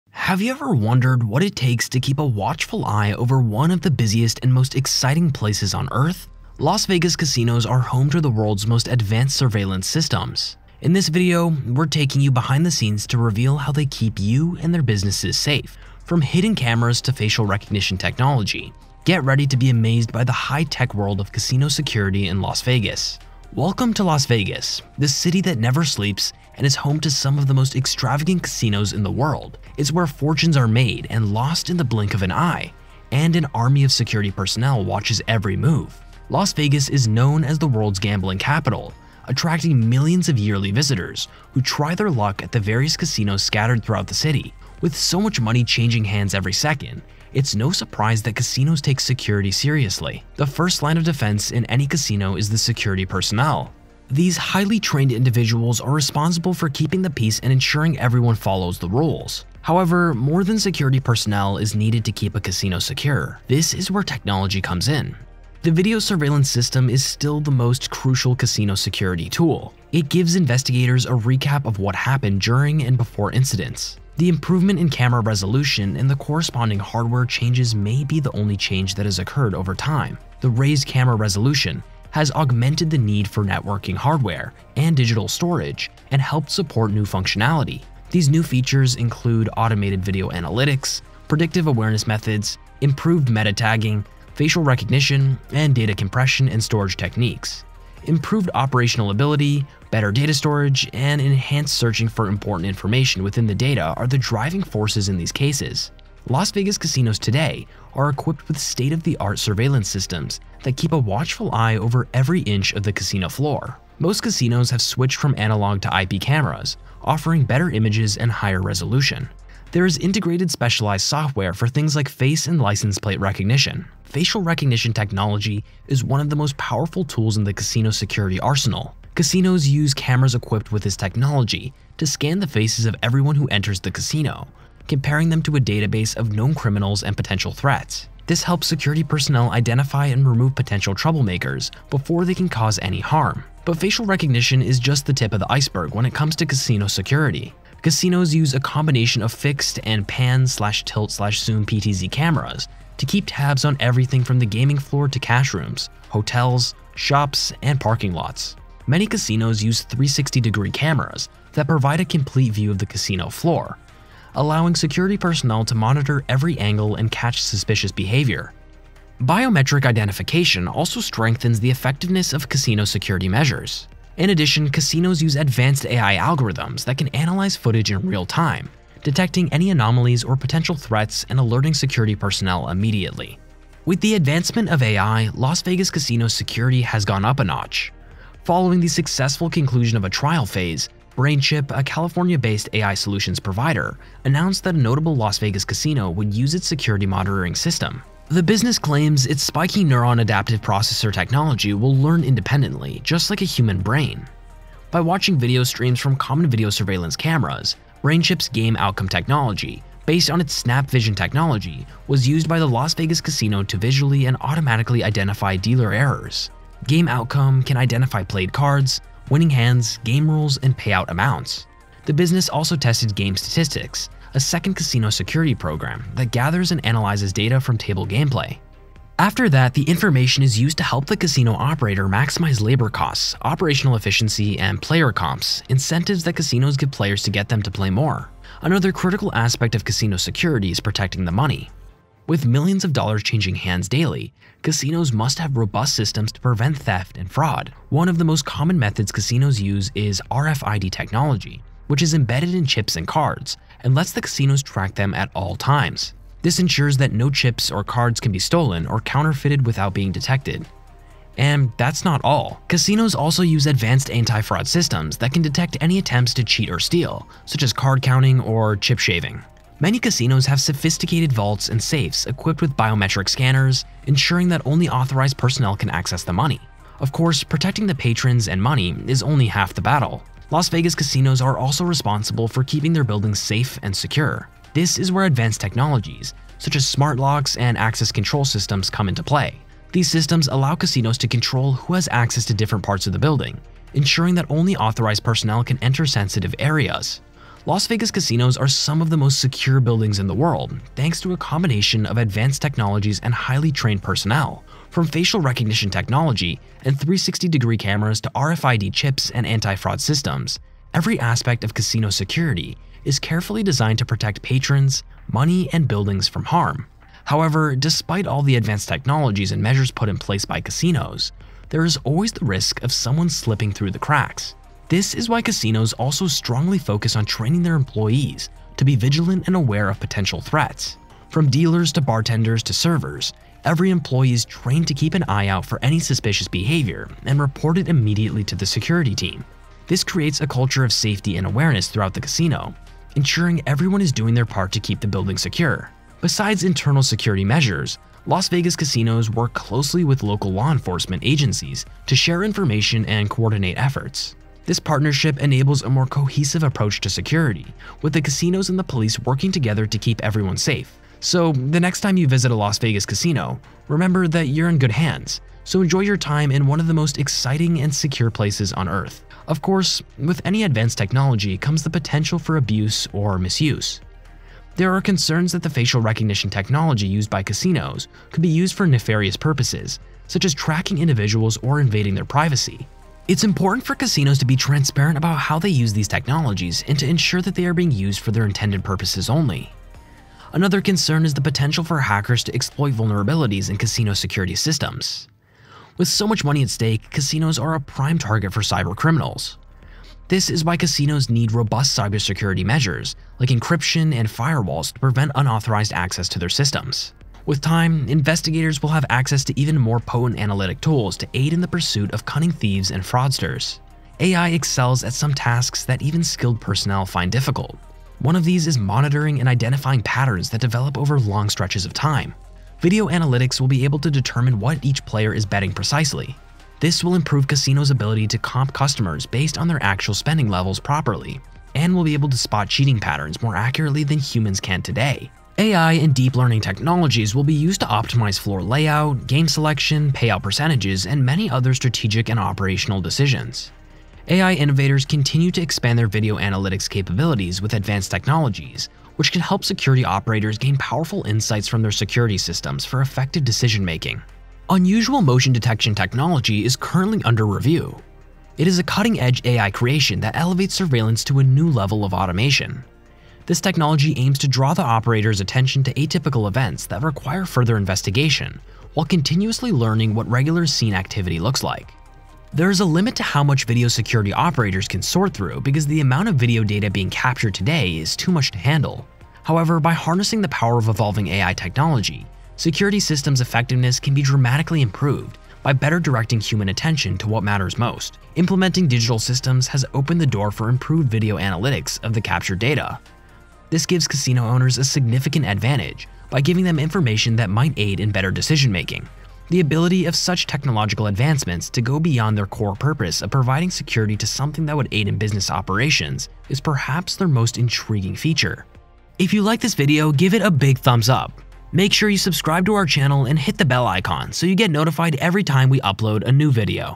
Secret Technology of Las Vegas Casino Security & Surveillance
Have you ever wondered what it takes to keep a watchful eye over one of the busiest and most exciting places on earth?
Las Vegas casinos are home to the world's most advanced surveillance systems.
In this video, we’re taking you behind the scenes to reveal how they keep you and their businesses safe.
From hidden cameras to facial recognition technology, get ready to be amazed by the high-tech world of casino security in Las Vegas.